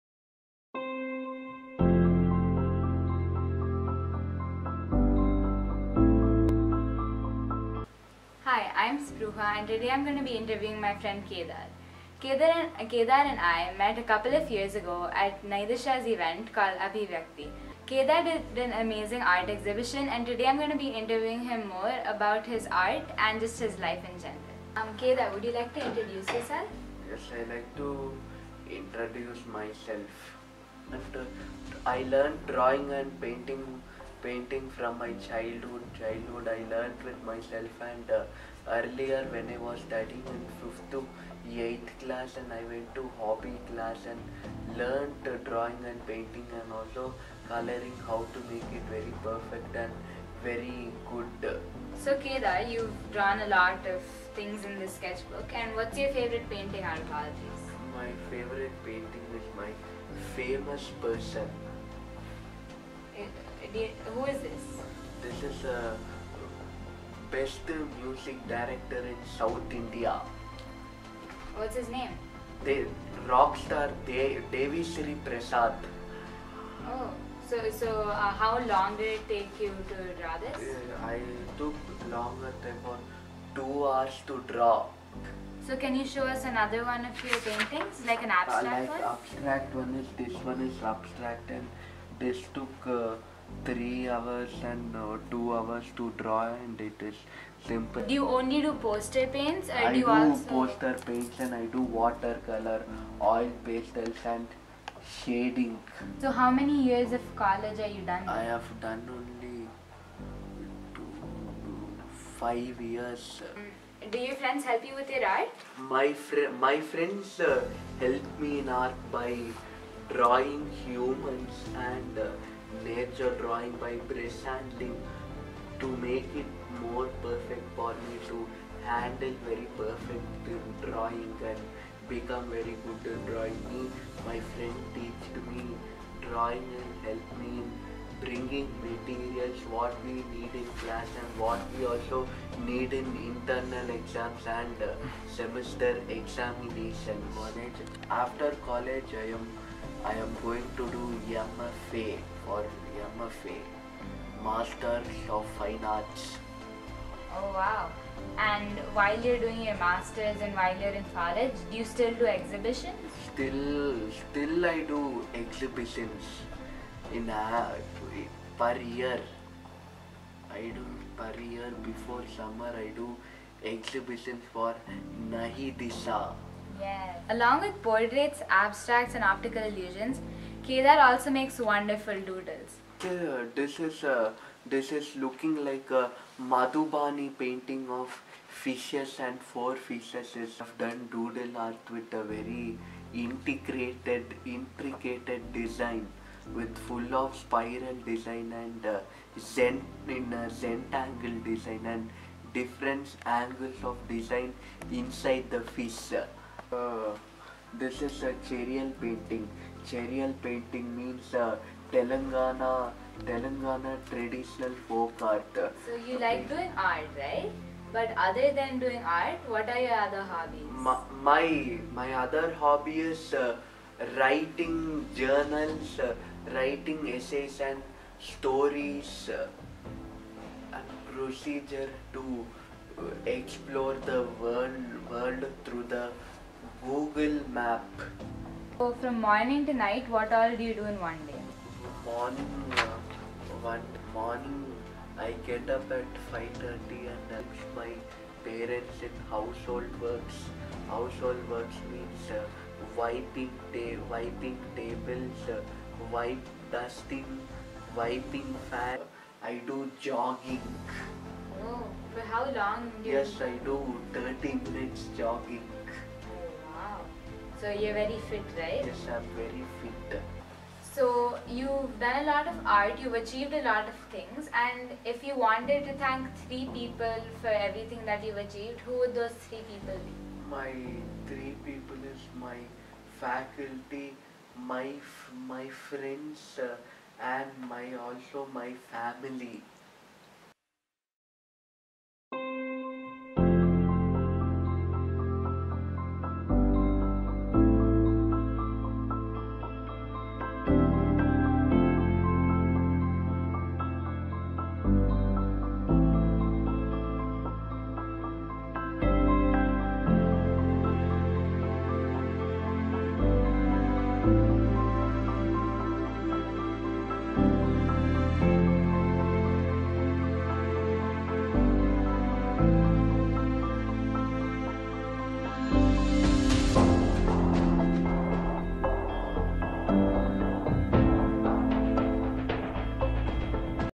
Everyday Heroes | Kedar, The Artist | Nayi Disha Resource Centre
Kedar is Nayi Disha Resource Centre’s Everyday Hero!
With a supportive family & consistent therapies, he is today a prolific artist who showcases his spectacular work through annual exhibitions!
Godspeed, Kedar!